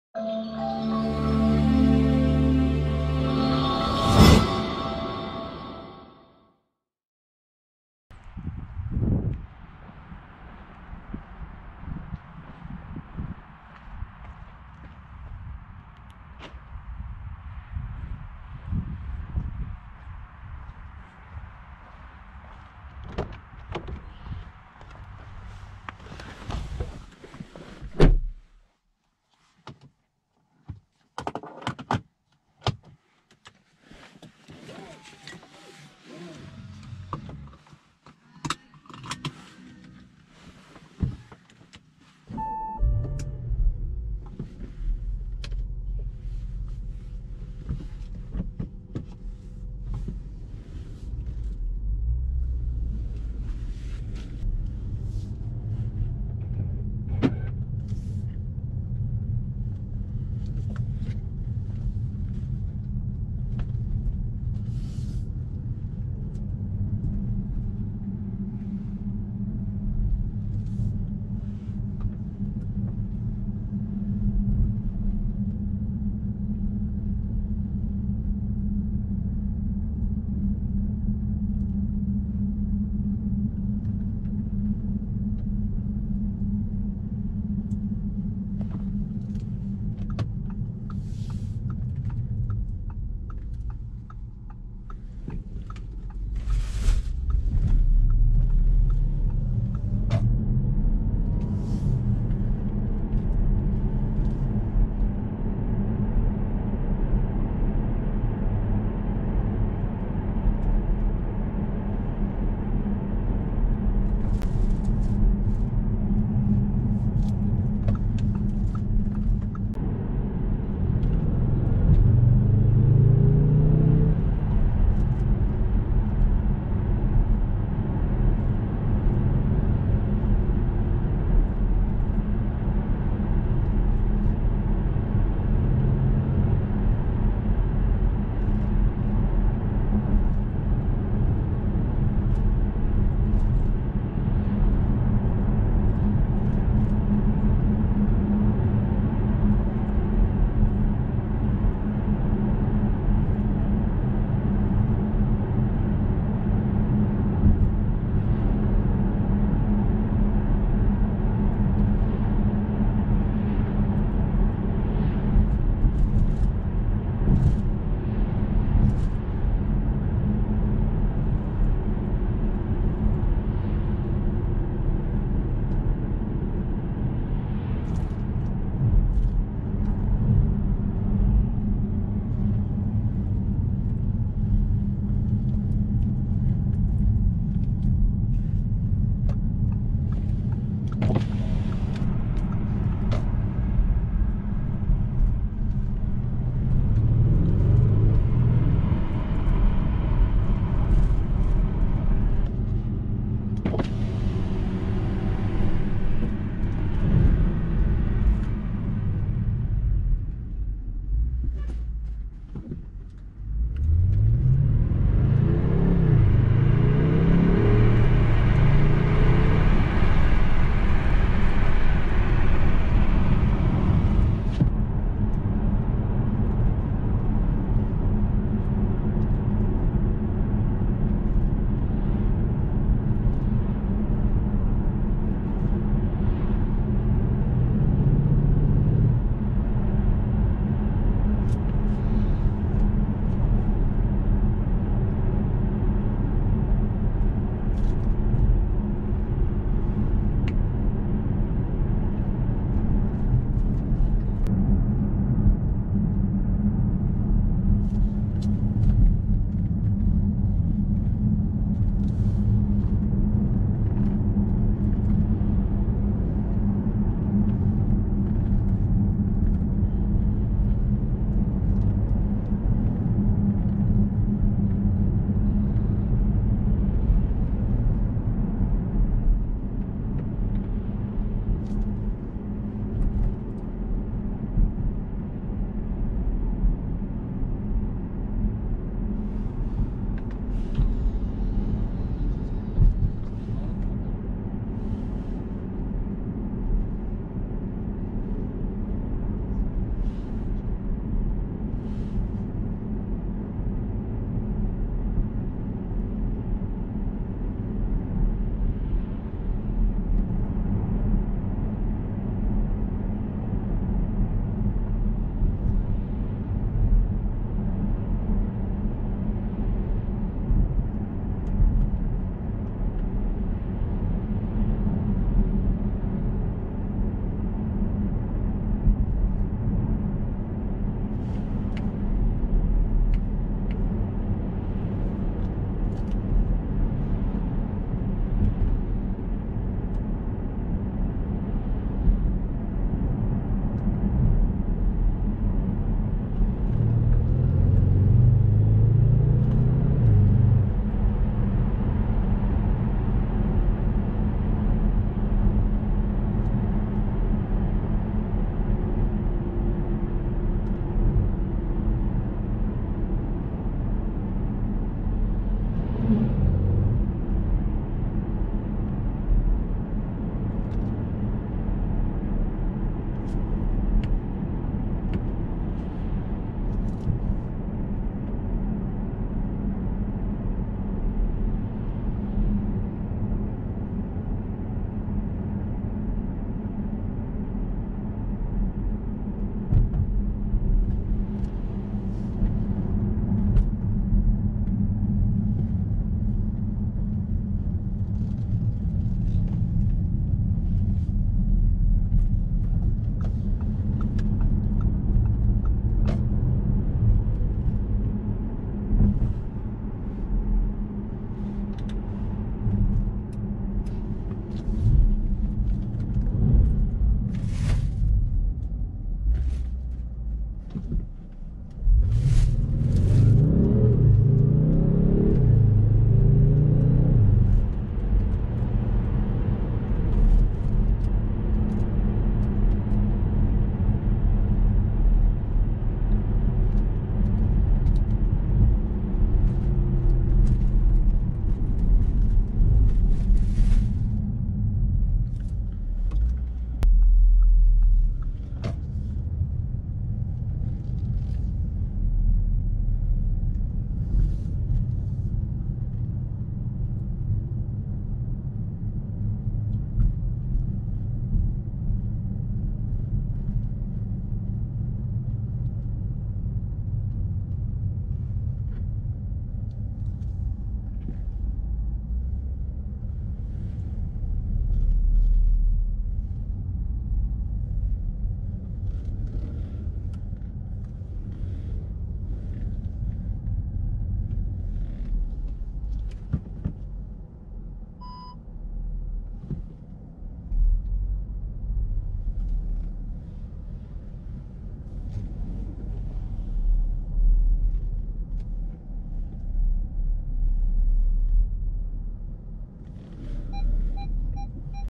2014 AUDI S6 Avant 4.0 TFSI V8 [POV Drive] // AutoDriverTV
[ENG]
POV Drive of an AUDI S6 C7 Avant 4.0 TFSI V8 quattro with 420HP and 550NM
The car was provided by Autowelt von Rotz AG, hit the

[DE] POV Fahrt von einem AUDI S6 C7 Avant 4.0 TFSI V8 quattro mit 420PS und 550NM
Das Auto wurde zur Verfügung gestellt von Autowelt von Rotz AG

My Cam